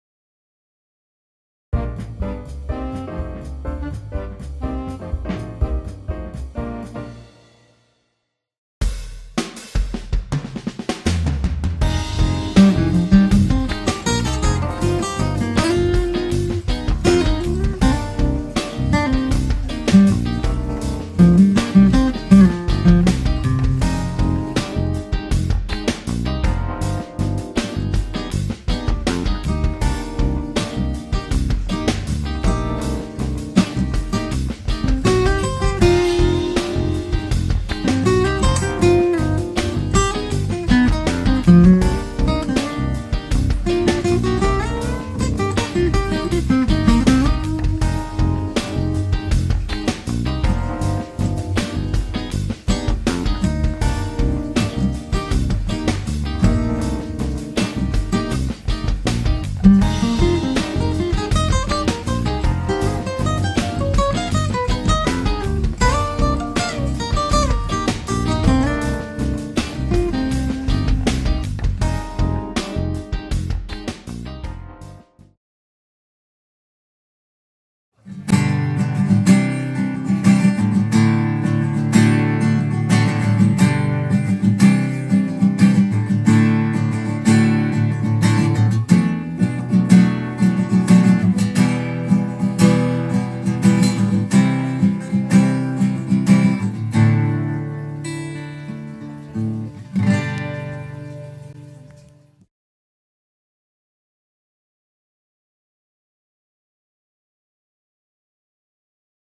Compare between Dreadnought and Grand Concert Acoustic Guitar shape (Part Two: Grand Concert)
What Acoustic Guitar Body Shape that FITS you? This video helps you compare between Dreadnought and Grand Concert shapes that affect the sound that will be produced by the guitar
In this video, the second part of the two video series, you will hear the sound produced by the Grand Concert shape

Both of these guitars were recorded using the same recording device, recorded sequentially.

Both of these guitars are made with the same material and also use the same parts

Cheers,
Robby Ramdhani